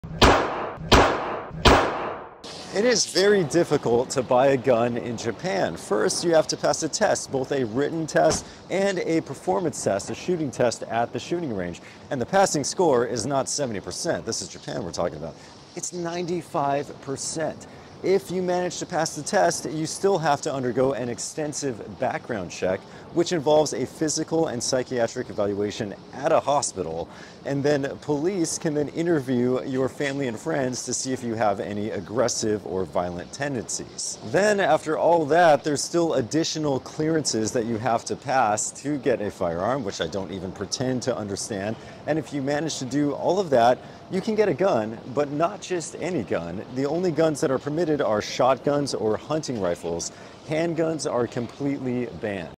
Gun Control in Japan
Different countries have different laws when it comes to who can own a gun and also what type of guns are permitted.
X
Perhaps the starkest contrast with regard to this is Japan versus The United States. 🇯🇵 🇺🇸
X
The two nations have very different attitudes when it comes to firearms.
X
What do you think about gun control laws?  Should they be strict as in the case with Japan or more lax like what we have here in the States?